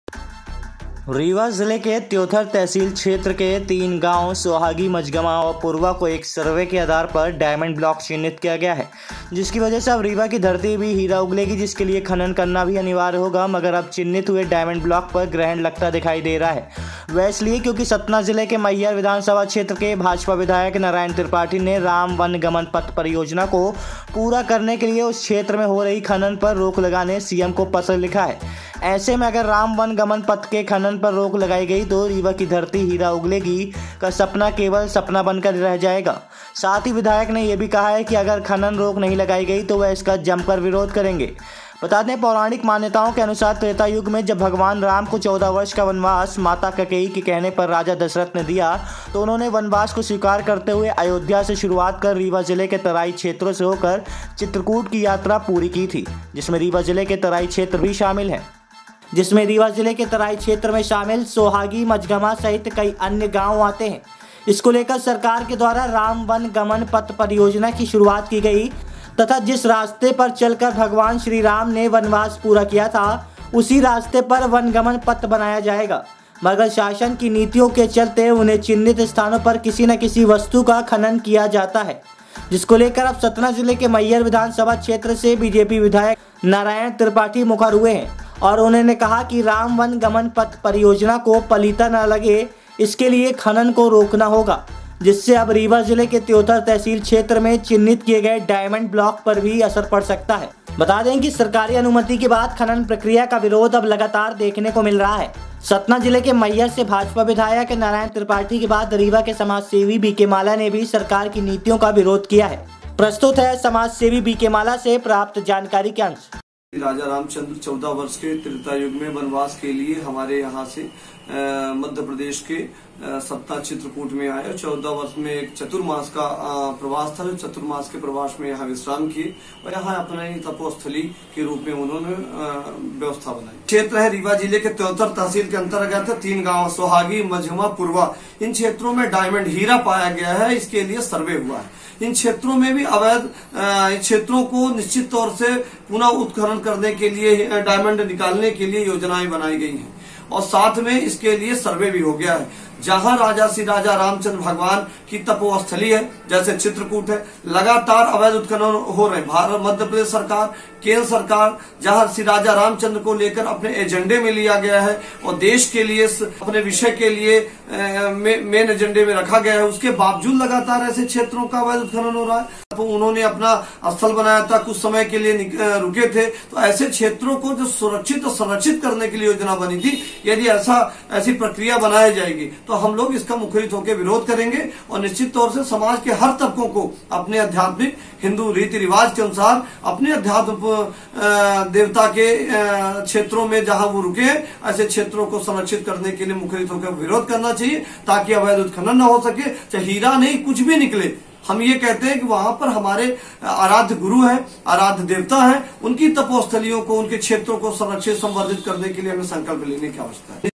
जिस रास्ते से गुजरे प्रभु श्रीराम वहा हो रही खनन की तैयारी..चिन्हित डायमंड ब्लॉक हो सकता है प्रभावित
*राम गमन पथ परियोजना: जिस रास्ते से गुजरे प्रभु श्रीराम वहां हो रही खनन की तैयारी.. रीवा में चिन्हित डायमंड ब्लॉक हो सकता है प्रभावित,,,,,देखें सिटी न्यूज़ वैन रीवा की ये खबर।*